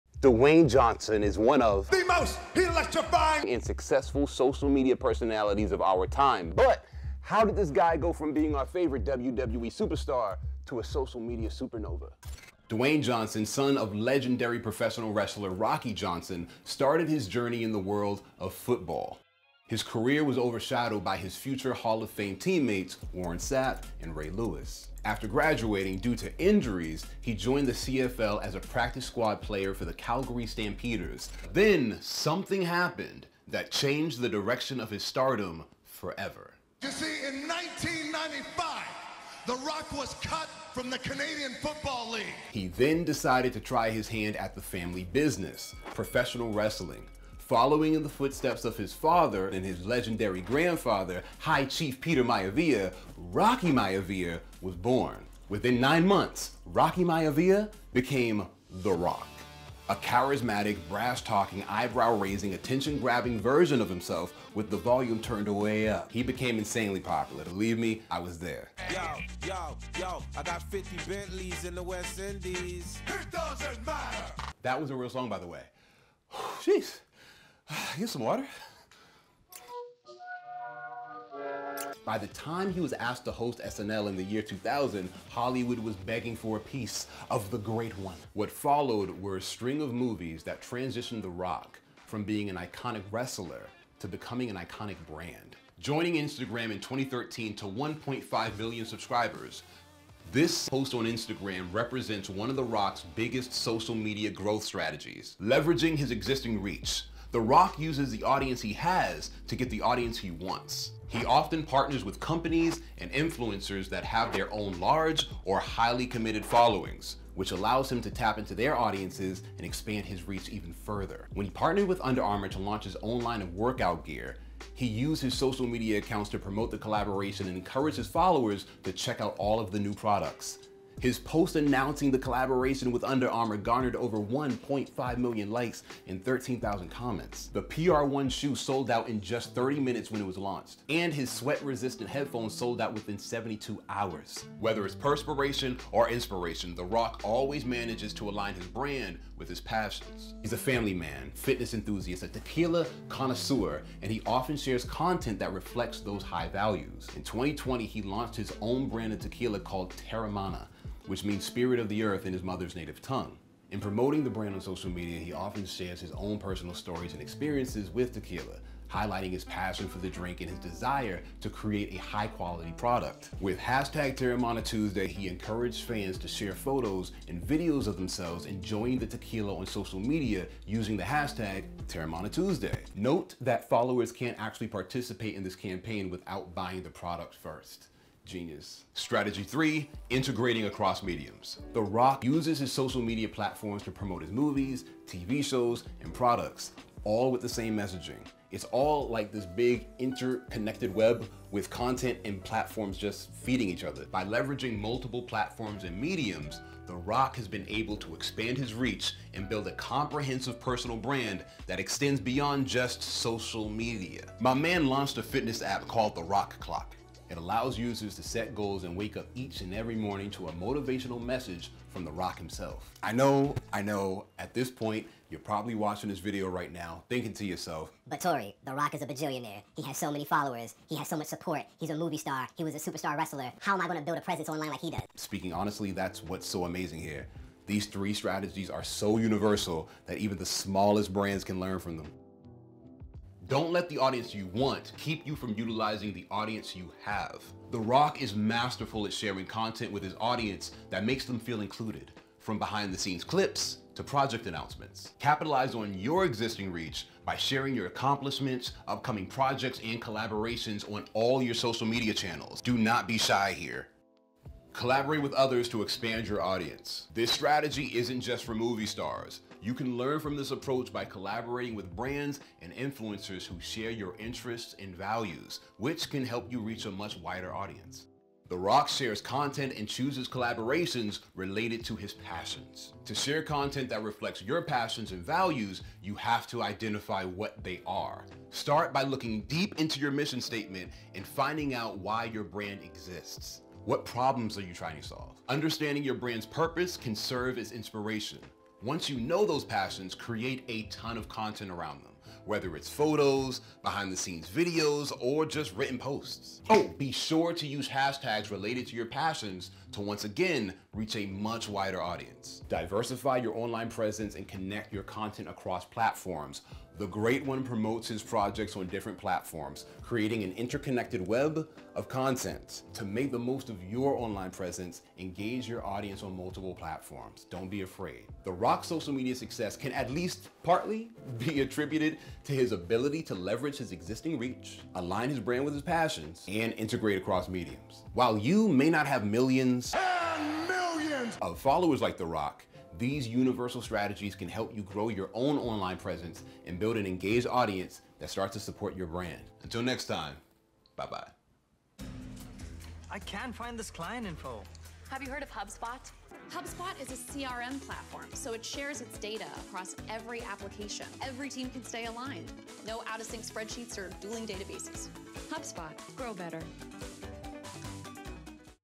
How The Rock Dominated Social Media (Growth Tips YOU Can USE)
In this video, we take a look at how Dwayne “The Rock” Johnson single-handedly dominated social media - building an online personal brand that is unrivaled in the entertainment industry. Even if you're not in the entertainment industry, there are still some great takeaways for you to apply to your own personal branding efforts.

📔 Favorite Free Certification Courses:
• Digital Marketing Certification: